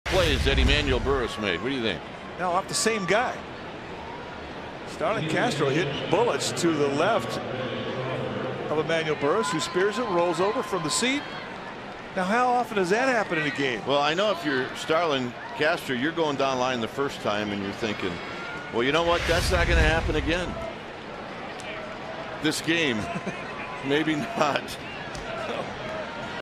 2011/06/30 Burriss' identical plays
SF@CHC: Burriss makes two identical plays on Castro